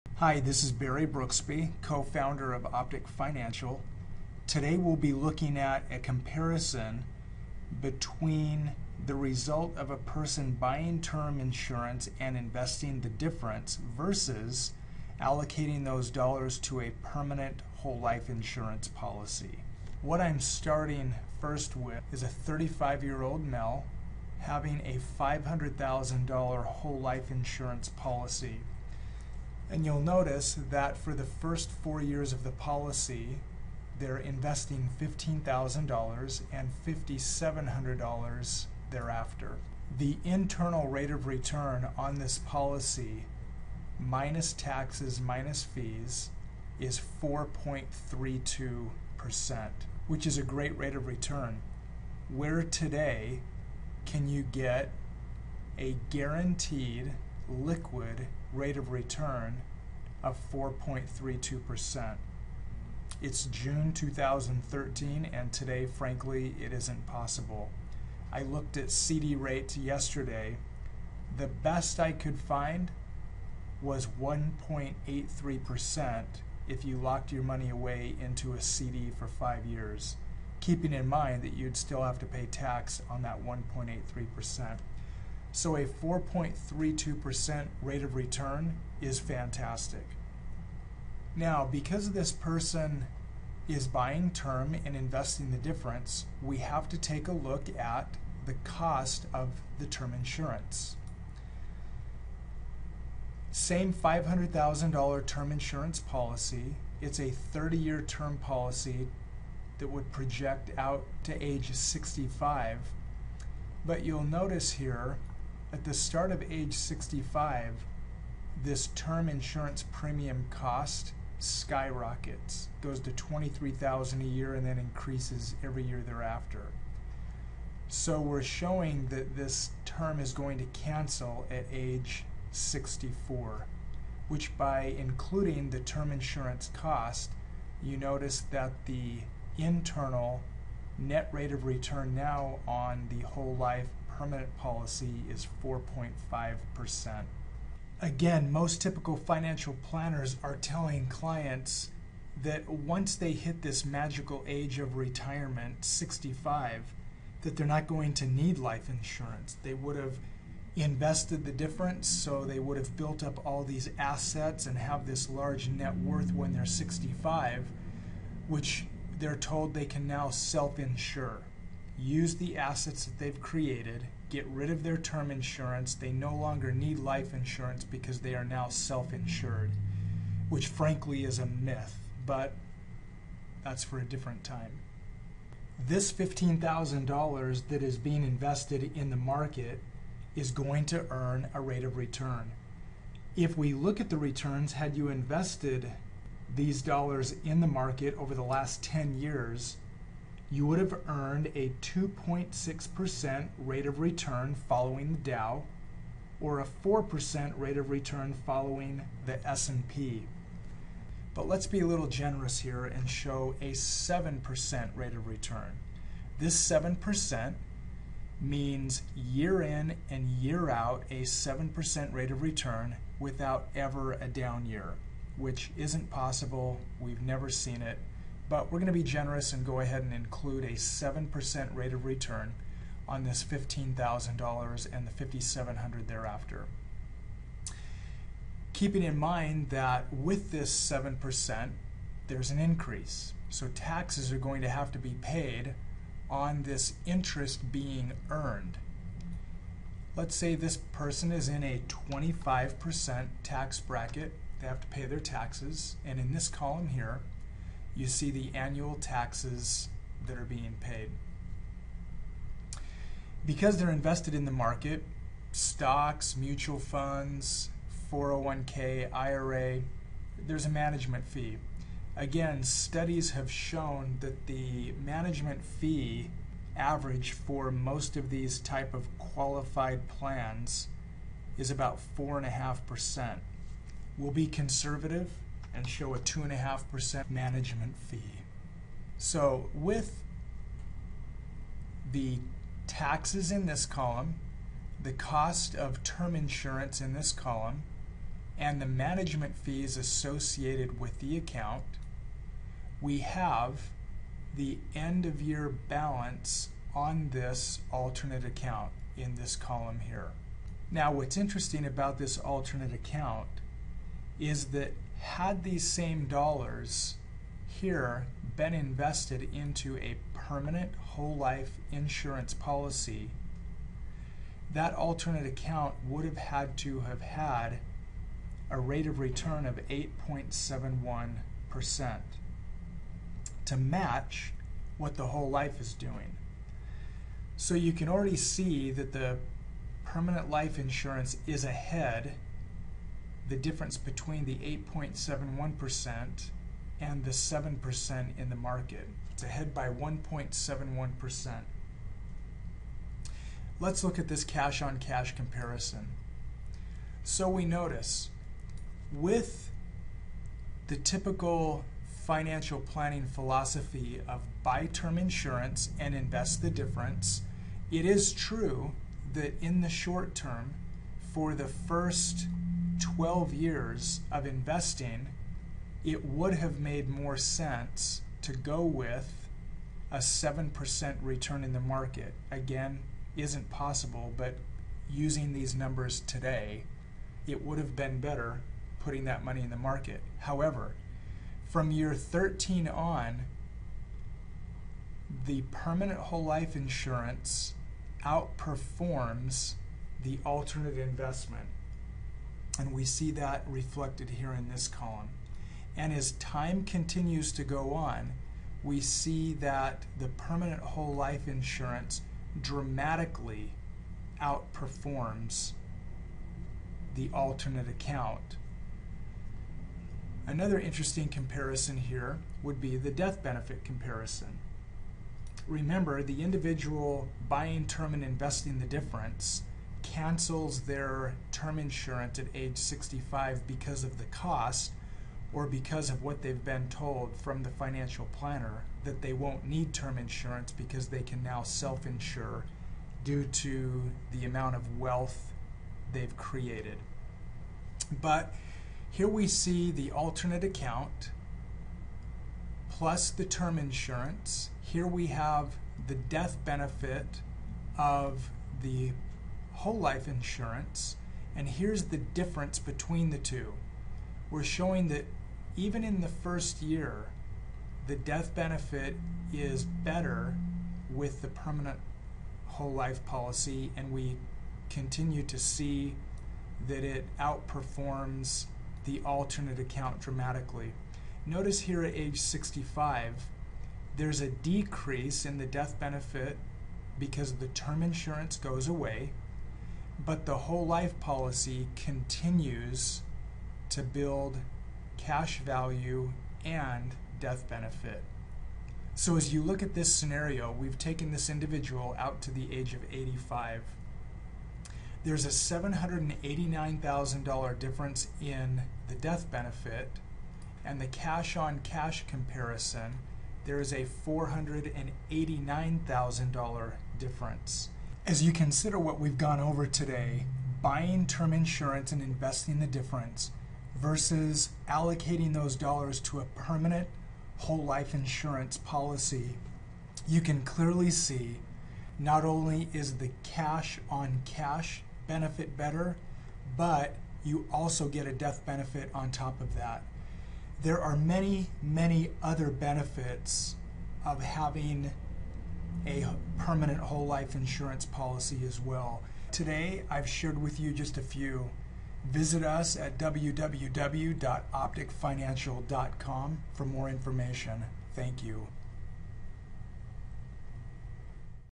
Buy term insurance and invest the difference, is this the road to wealth?
Traditional Financial Planners recommend that we buy term insurance and invest the difference in the stock market. However, more times than not, this advice does not work. See how in this one example a Cash Value Whole Life policy can increase your wealth and assets beyond the traditional advice of buying term and investing the difference.